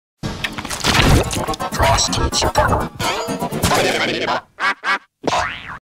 (Back on my computer) Klasky Csupo in FNF Pico Effect
• Apply HSL Adjust and edit:
        Add to hue: 0.66
        Saturation: 1.00
        Luminance: 1.00

• Apply Invert and select 100% Inversion

⸻ Audio ⸻

• Pitches:
       +3.50
       -3.50

Made in Sonic Foundry Vegas 4.0

and all the TPTMC1756 logos